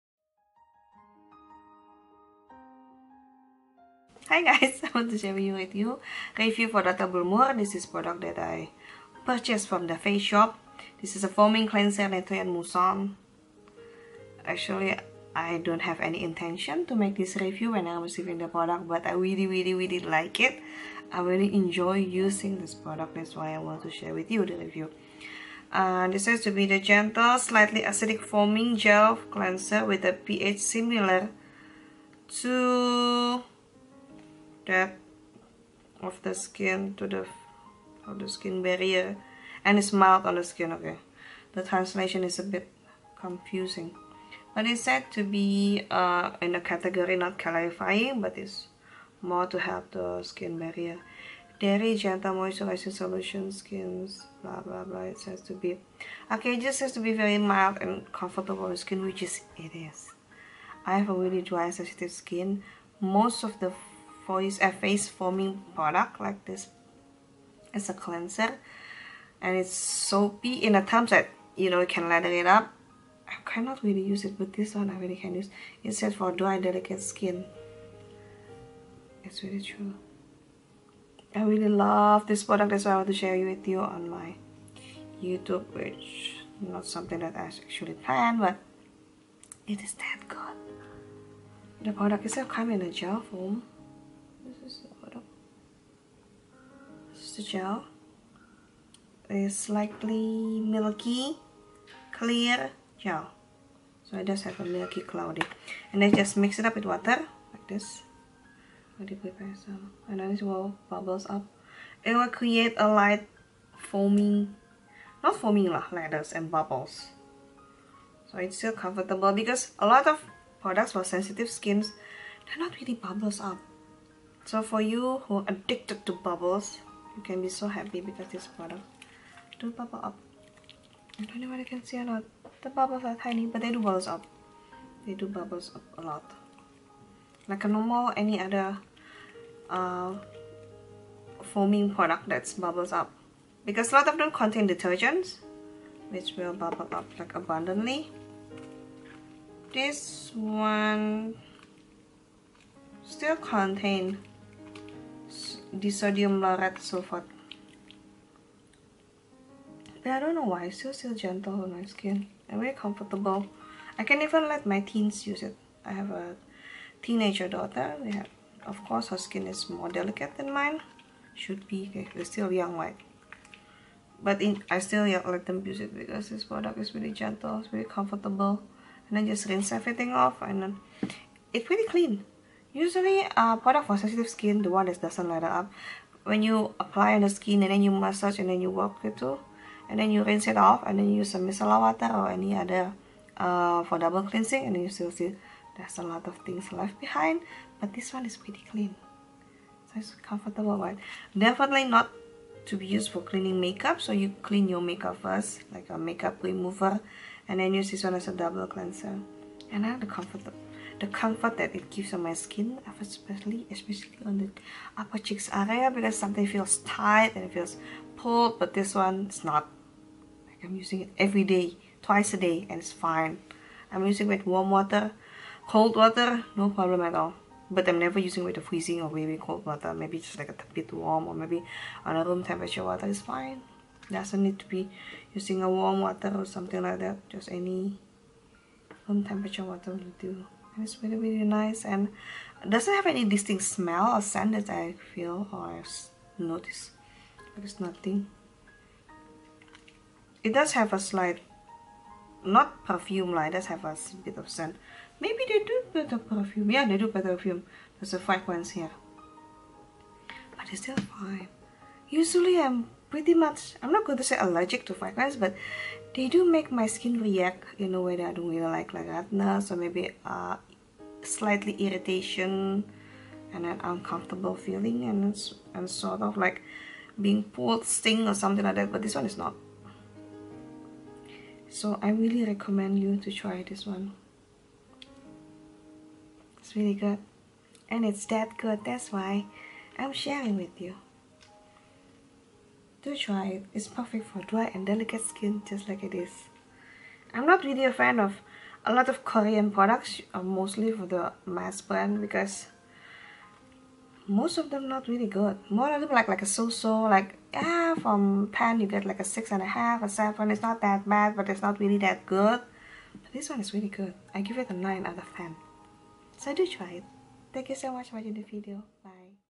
Dr. Belmeur Daily Repair Foam Cleanser
A product from The Face Shop that cleanse sensitive and dry skin without damaging